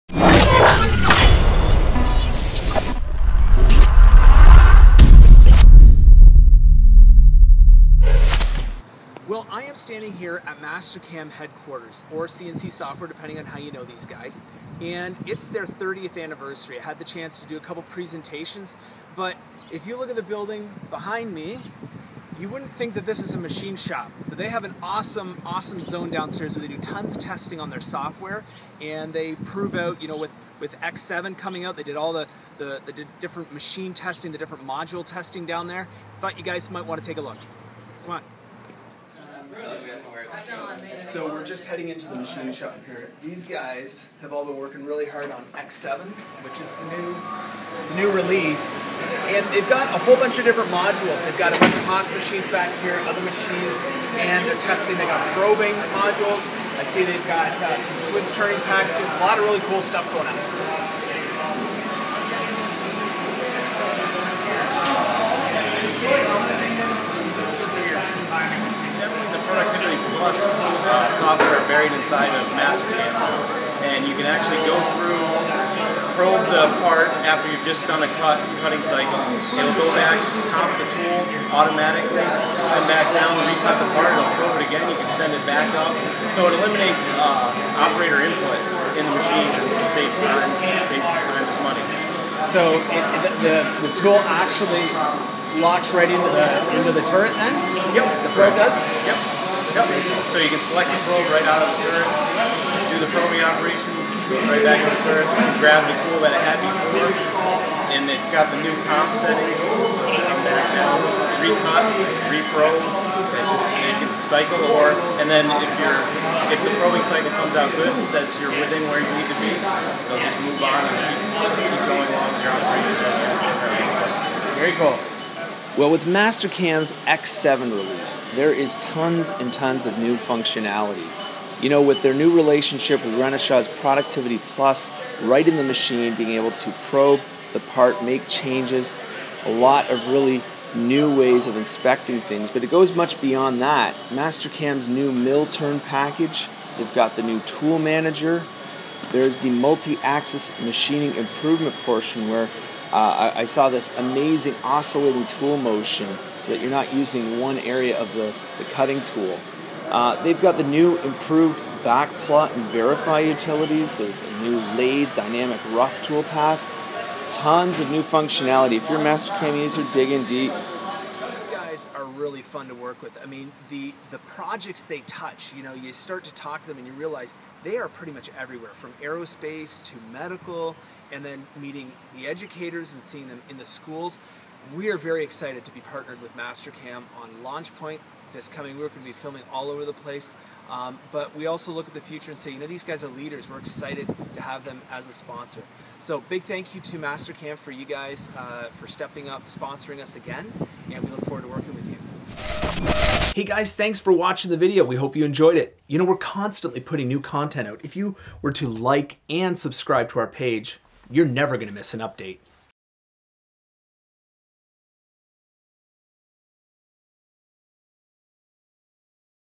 Edge Factor: Behind The Scenes: Mastercam HQ
Jeremy Bout visits the Mastercam Headquarters! Learn more about some of the technology, machines at their shop!